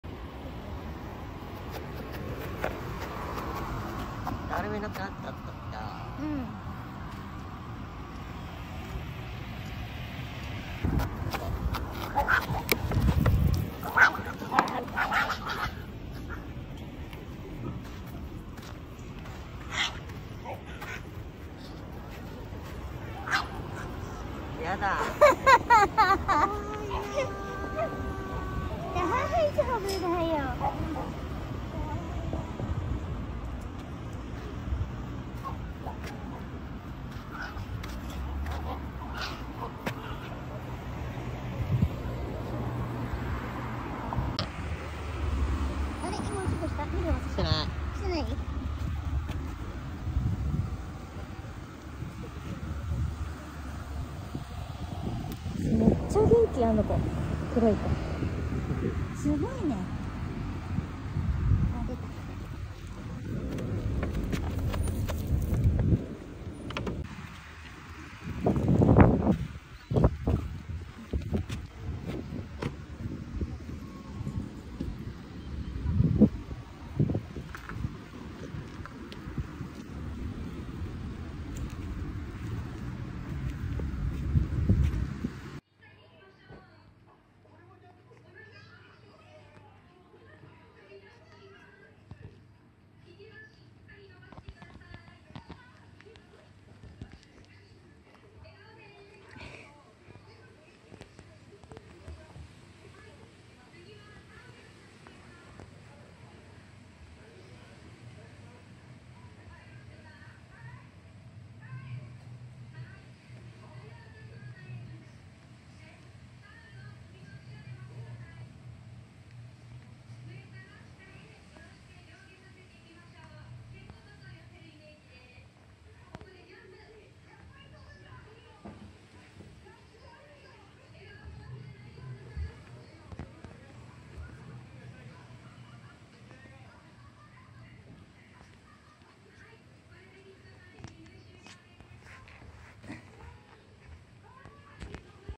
久々にアップ ポメラニアンぽんちゃんのドックランからの1コマ。
久しぶりアップします。12月は出来る限りの動画大量アップします🙏

ポメラニアンぽんちゃん1歳と8ヶ月。

【ニコ生】
【17Live】
【ふわっち】